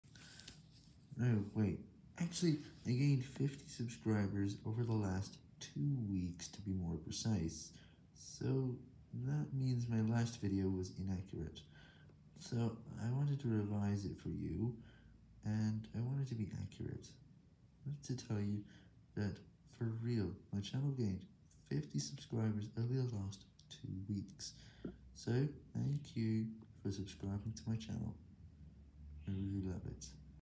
I gained 50 subscribers in the last 2 weeks.
I have to record this two times, as I thought the first one was inaccurate.